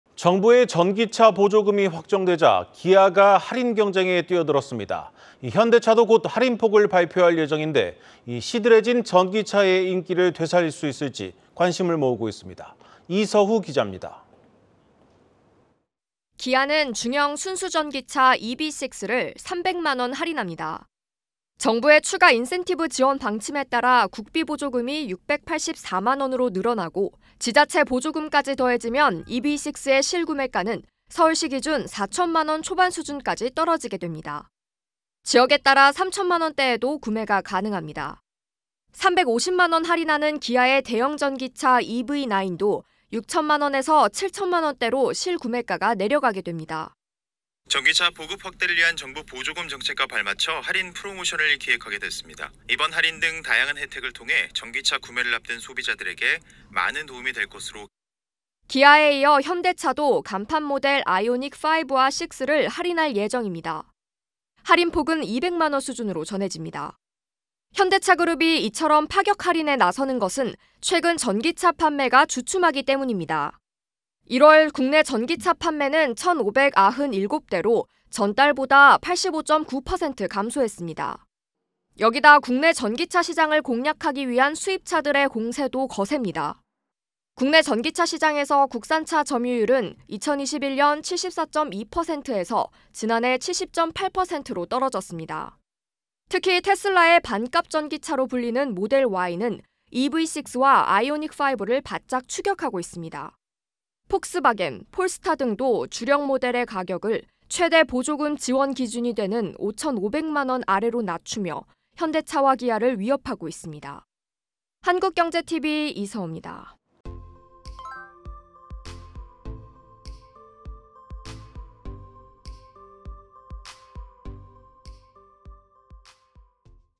EV6 3천만원대…아이오닉도 곧 할인/한국경제TV뉴스
정부의 전기차 보조금이 확정되자, 현대차와 기아도 전기차 할인 경쟁에 뛰어들었습니다. 대규모 할인만으로, 시들해진 전기차의 인기가 되살릴 수 있을 지, 관심을 모으고 있습니다. 산업1부 이서후 기자입니다.